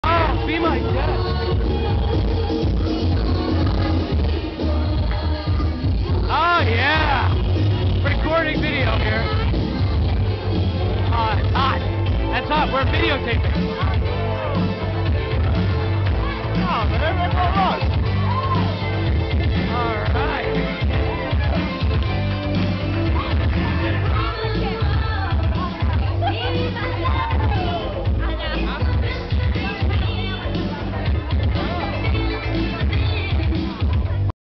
Our Wedding Reception Party Part 2
Our wedding reception party at the Poughkeepsie Grand Hotel on May 10th, 2008